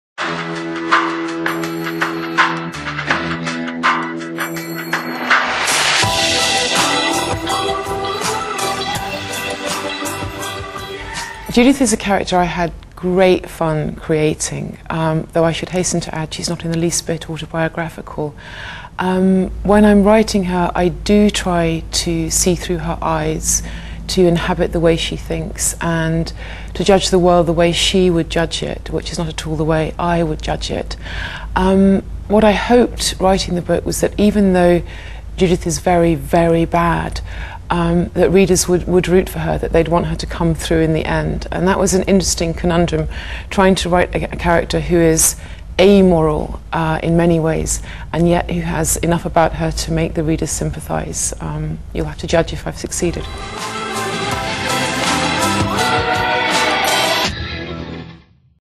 LS Hilton on Judith Rashleigh of Maestra
LS Hilton talks about her lead character of Maestra, the feisty femme fatale Judith Rashleigh.